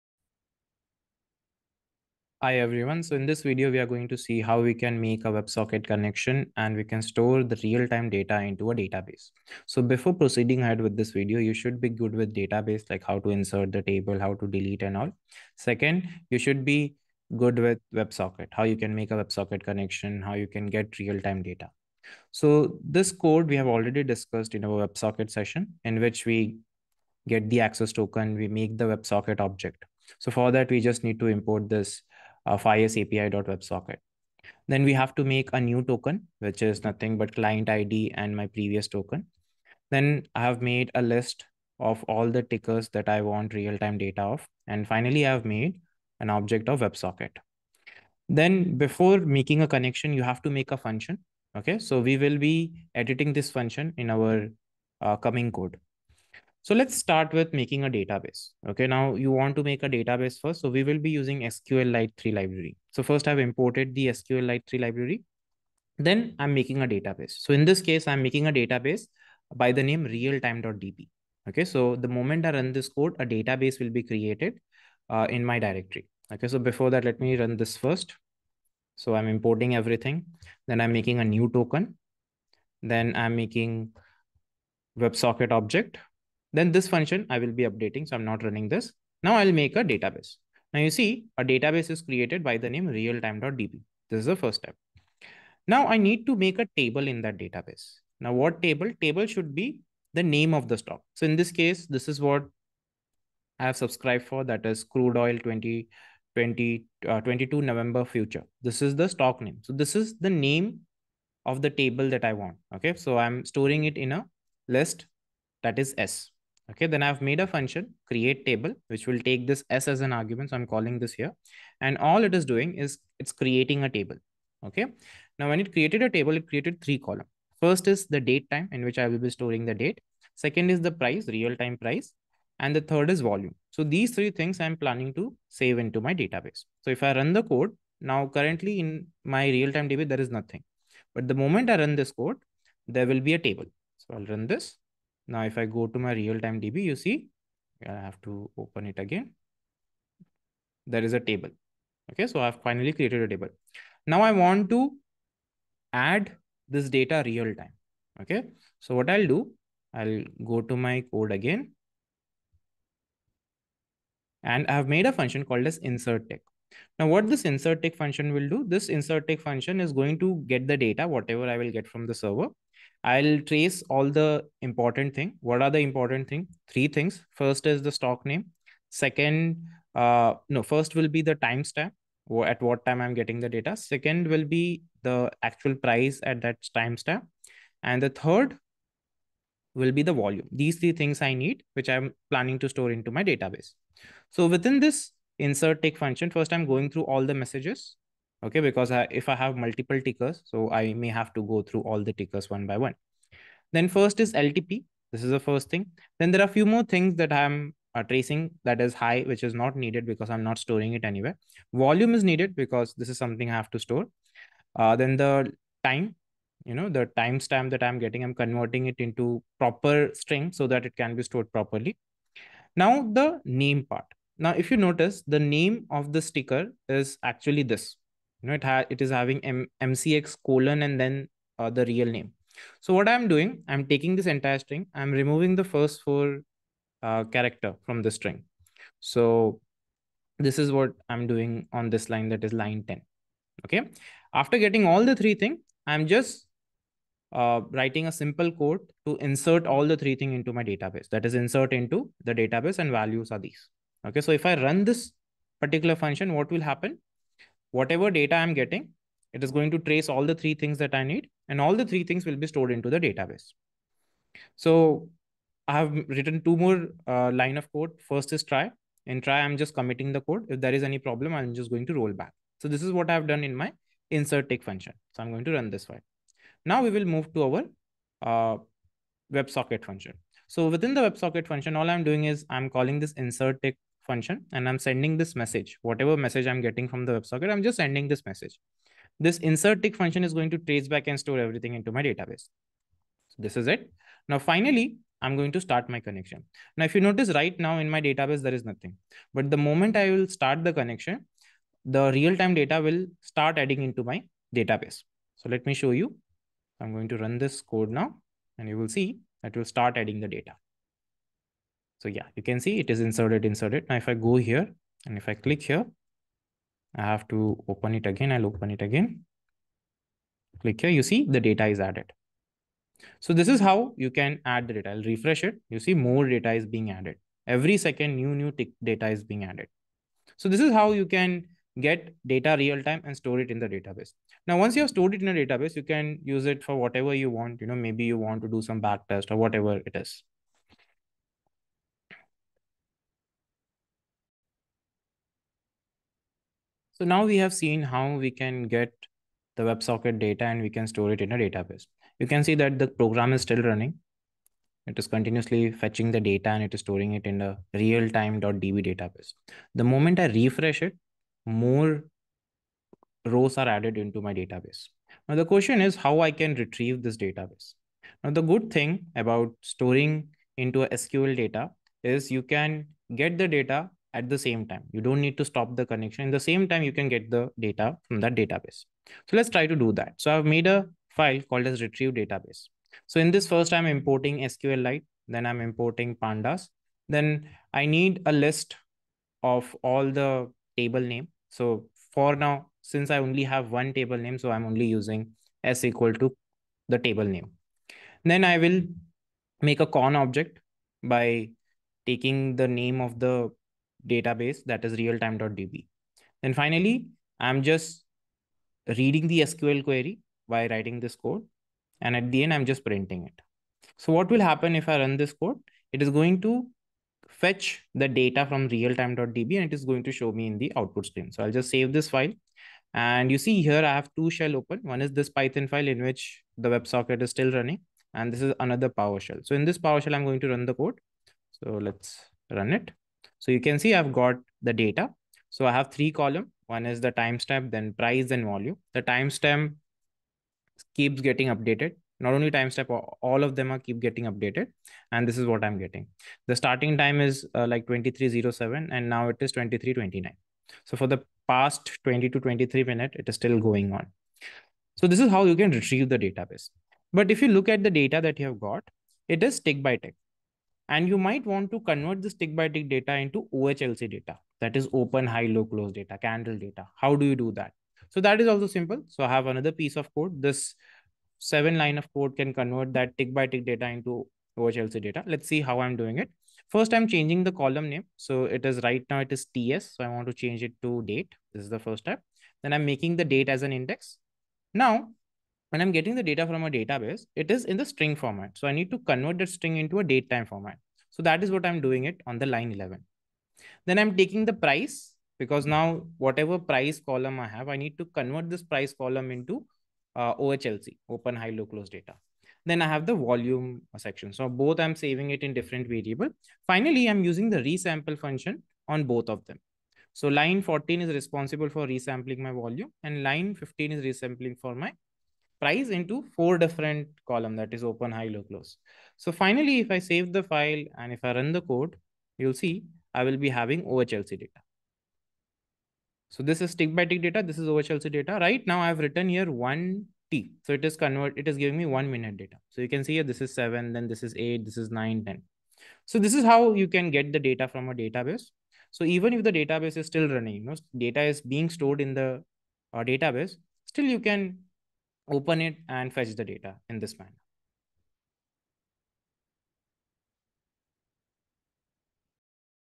Storing live data in database using python part 7
In this video we have seen how to make a database and then how to store the data realtime in the database

Book your spot for the summit 2022